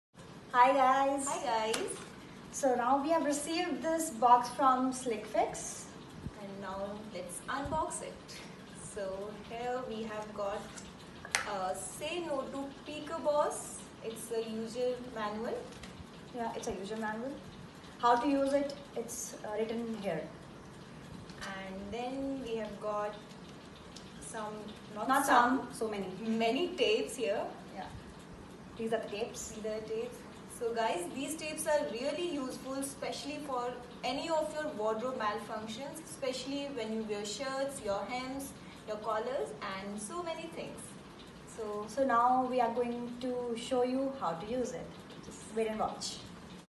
Kritika & Shalini unbox Slickfix Fashion Dressing Tape
Here's Kritika & Shalini (@tripbeees) unboxing Slickfix Fashion Dressing Tape. Follow them on instagram for more product reviews.

About the product: Fashion Dressing Tape is a double-sided clear tape which can be used with any fabric without leaving a stain on the garment. The strength of the adhesive is just right to last you the entire workday or a long party night while being mild enough to suit all skin types.

Available in a monthly and a super saver pack this is a must-have in your bag for all fashion emergencies.